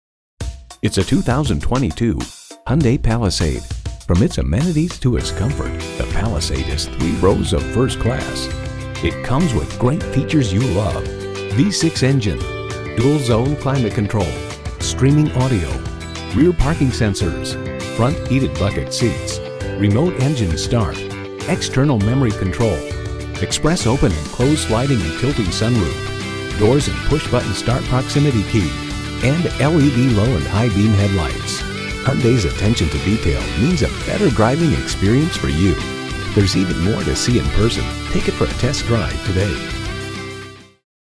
Used 2022 Hyundai Palisade Minneapolis MN Eden Prairie, MN 211447B8 - SOLD
Year: 2022
Make: Hyundai
Model: Palisade
Trim: SEL FWD
Engine: 231 L V6 Cylinder Engine Cyl.
Transmission: 8-Speed A/T
Color: HYPER WHITE
Interior: Gray/Black
Mileage: 13239
Stock #: 211447B8
VIN: KM8R44HE1NU371652
CARFAX 1-Owner. EPA 26 MPG Hwy/19 MPG City! SEL trim.

Address:
12477 Plaza Drive